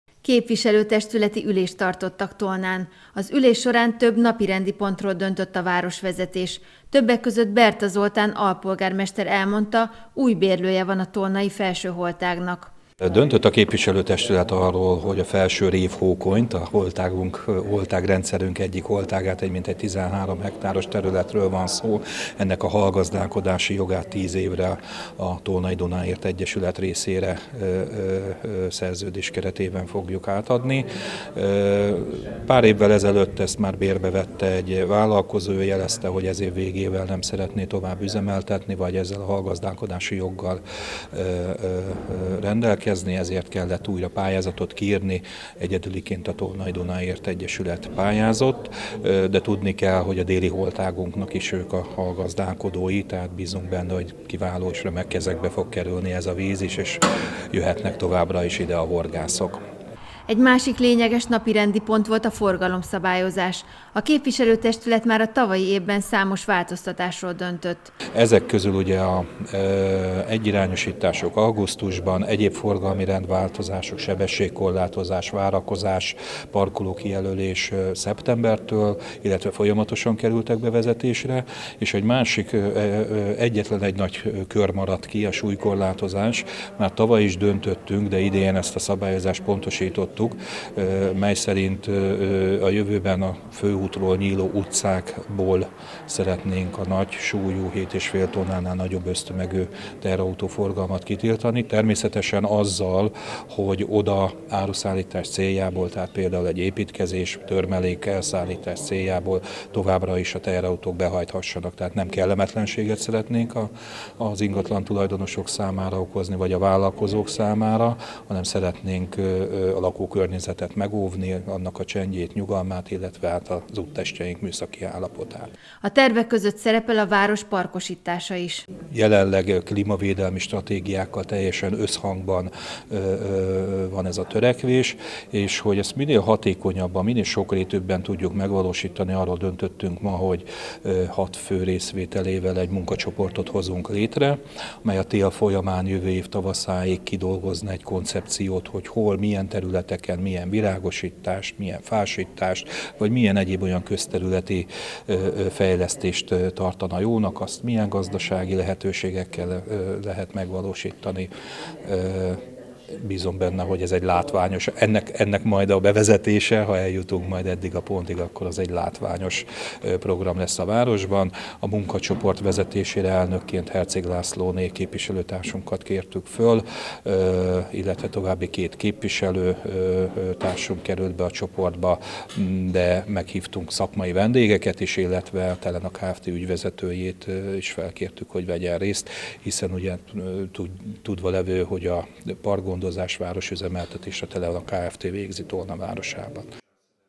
Tolna zöldterületeinek növelése a cél
A tolnai önkormányzati testület ülésén többek között arról hoztak döntést, hogy a jövőben súlykorlátozást vezetnek be a főútról nyíló utcákban. Továbbá a tervek között szerepel a város zöldterületeinek növelése is.